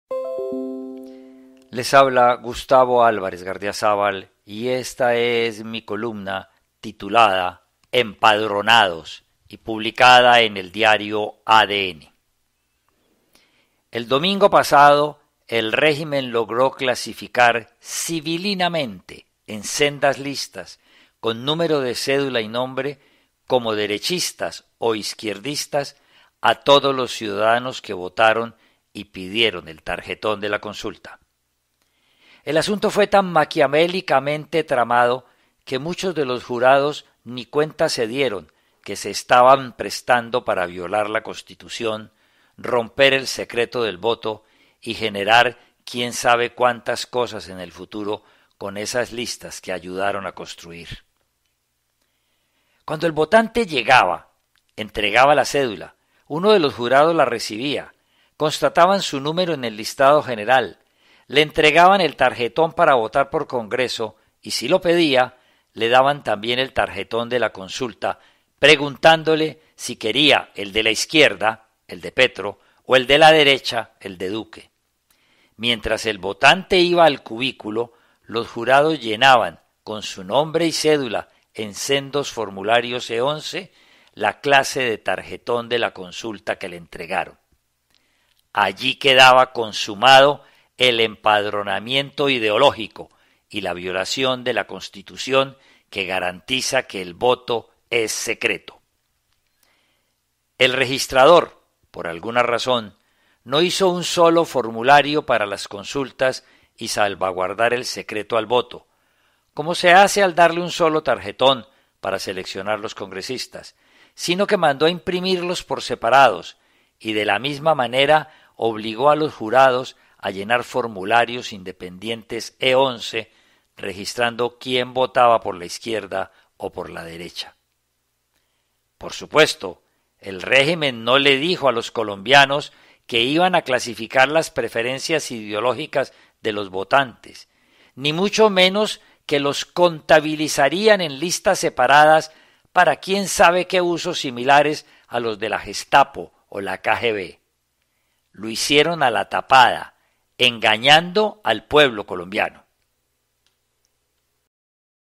EMPADRONADOS,Gardeazábal ADN, marzo 15 2018
Gardeazábal recoge la indignación nacional por haber sidos censados los votantes en las consultas del pasado domingo en listas separadas para los que pidieron el tarjetón de la izquierda y los que solicitaron el de la derecha